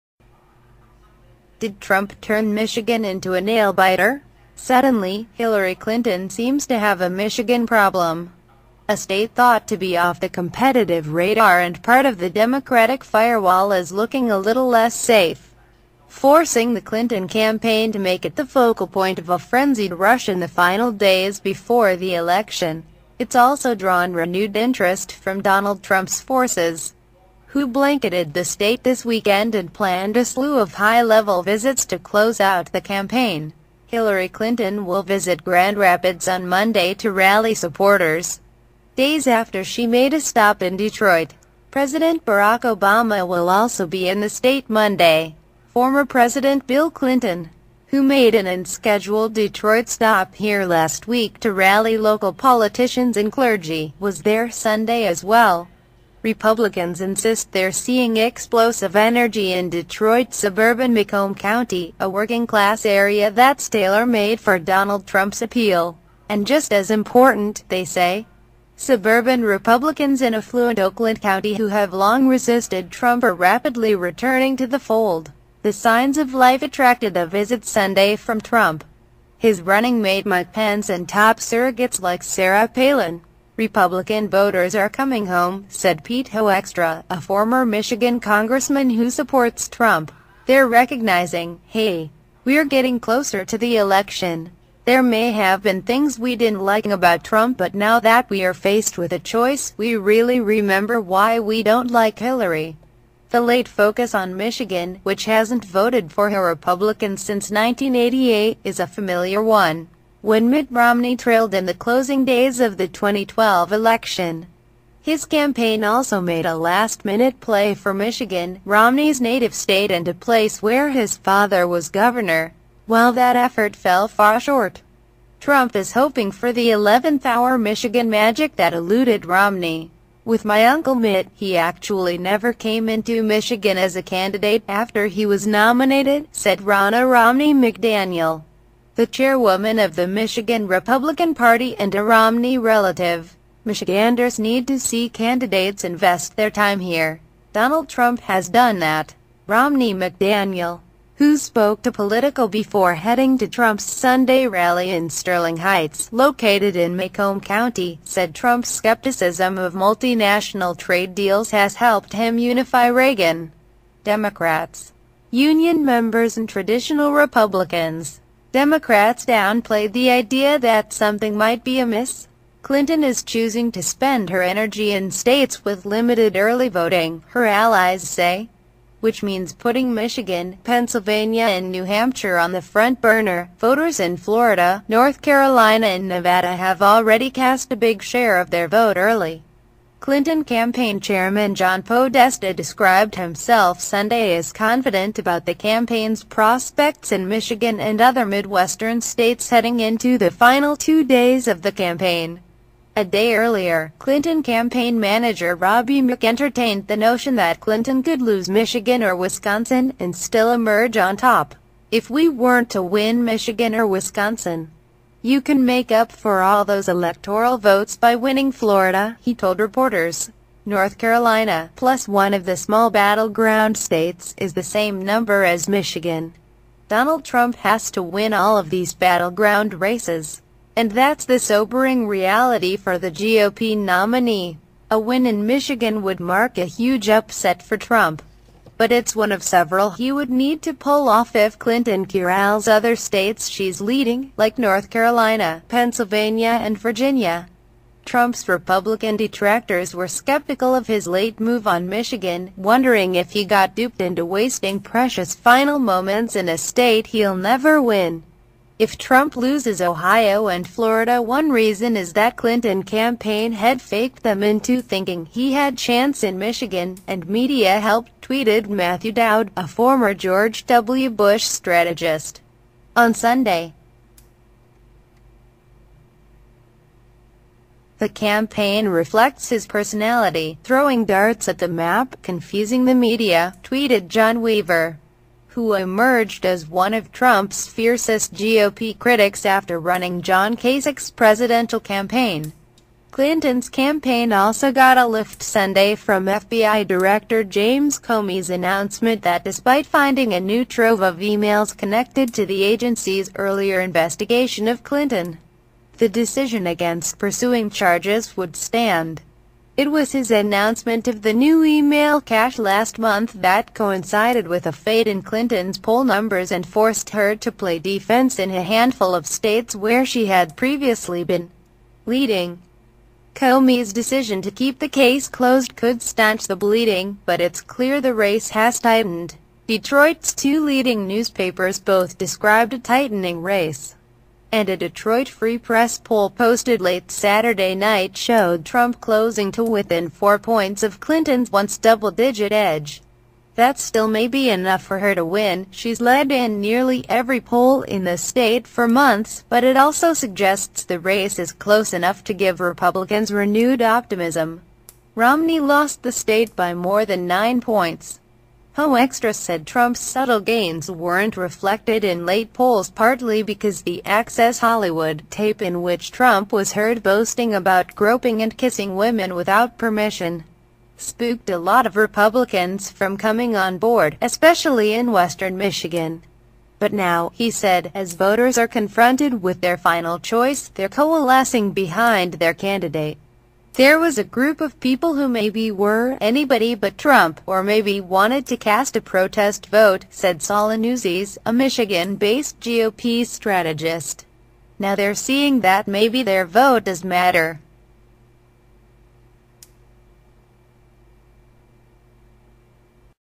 Did Trump turn Michigan into a nail-biter?
Suddenly, Hillary Clinton seems to have a Michigan problem.
A state thought to be off the competitive radar and part of the Democratic firewall is looking a little less safe, forcing the Clinton campaign to make it the focal point of a frenzied rush in the final days before the election. It's also drawn renewed interest from Donald Trump's forces, who blanketed the state this weekend and planned a slew of high level visits to close out the campaign.

Hillary Clinton will visit Grand Rapids on Monday to rally supporters, days after she made a stop in Detroit. President Barack Obama will also be in the state Monday. Former President Bill Clinton, who made an unscheduled Detroit stop here last week to rally local politicians and clergy, was there Sunday as well.